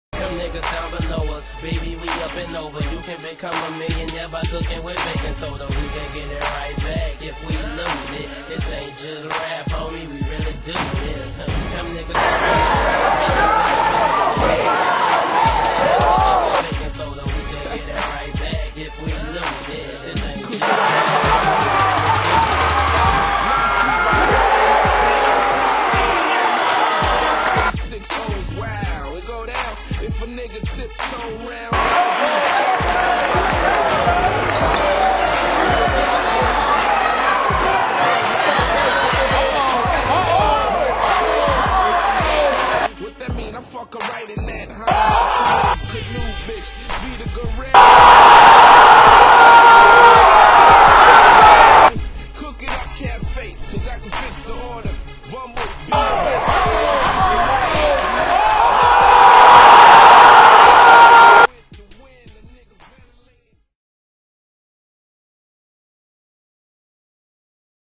Sims Frazier-Maury High School
Sims Frazier (#23) shows why no one in the Eastern District could check him his freshman year